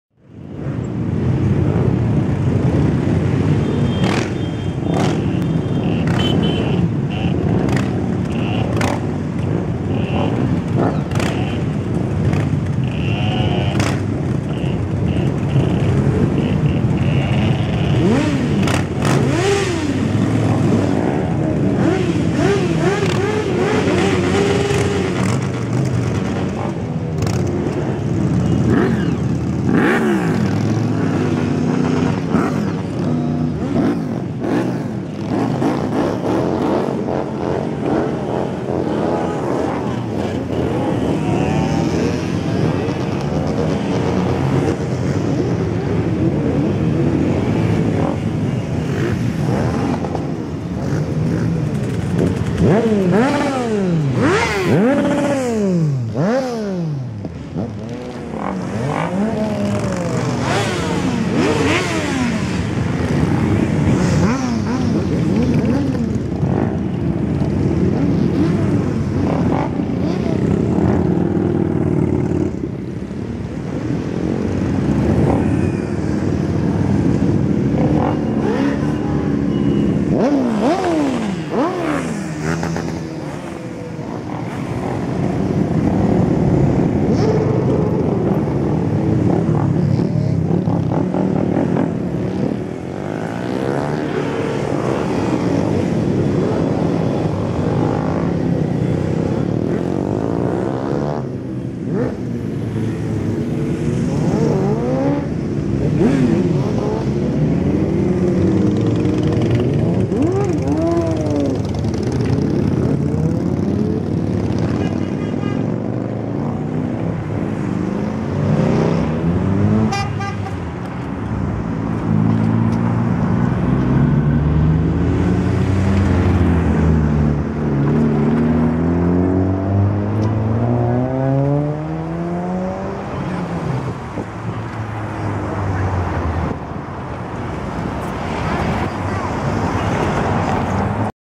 Nick Brisland Memorial Ride 2016 - Departure
I stumbled across this on a trip to Halfords, so I thought I'd film it. It was one year since 19 year old Nick Brisland died in a motorbike accident. This ride went to Portsdown Hill in his memory.

Note that the video ends after a few cars had left, I was a little confused and thought that was it. It actually took much longer to empty the car park of all of the cars which came along.